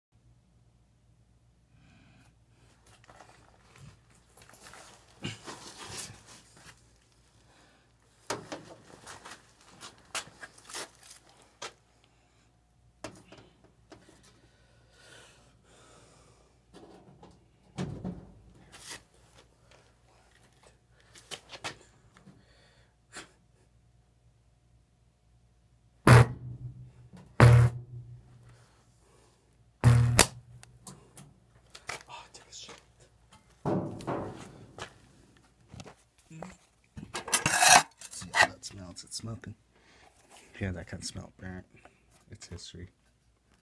Wall socketing a old woofer
Wall socketing a old woofer that is falling apart it was very brittle and when it got 120 volts it smelt very bad